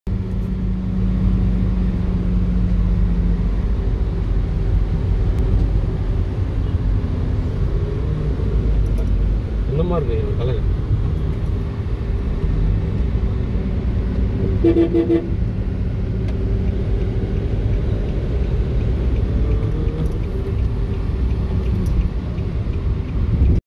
Sudden Rainbow Appears in Chennai Skies After Rain! 🌈✨
After a rainy spell in Chennai, something magical happened—a beautiful rainbow suddenly lit up the clouds, creating a stunning display in the sky! 🌧️☀️ Nature treated the city to this breathtaking moment, and it was nothing short of mesmerizing. The vibrant colors of the rainbow stretched across the sky, offering a perfect backdrop against the moody clouds and raindrops.

If you missed it, don’t worry—we captured this rare sight just for you! 🌈🌦️